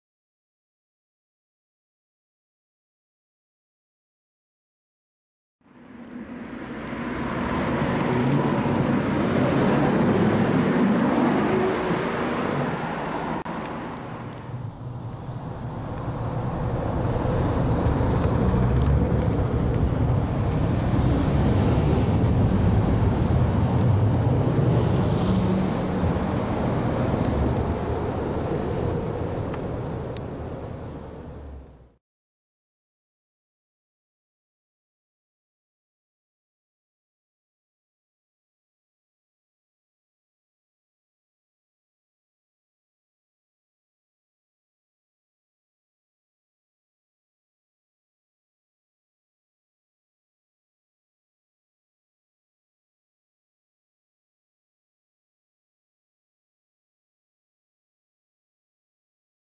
Porsche Panamera - 971-2 series - Barely Disguised Prototype - Part 9
The new Porsche Panamera - 971-2 series - barely disguised prototype caught in a district of Stuttgart (Germany).

This video is part 9.